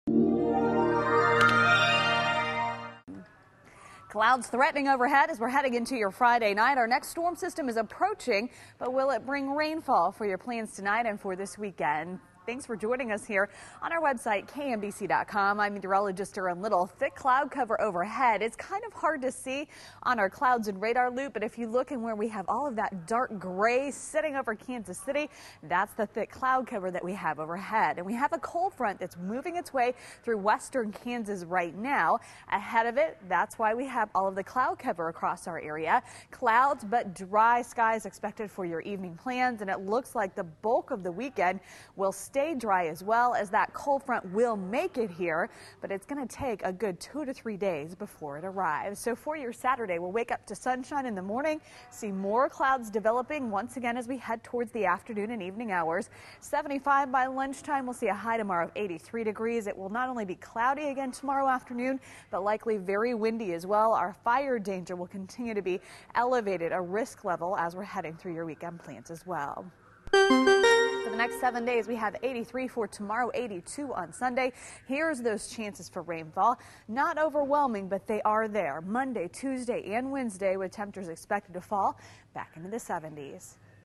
Warms Temps Extend Into Weekend
KMBC meteorologist Erin Little says the weekend will be on the warm side before a storm system approaches for the workweek.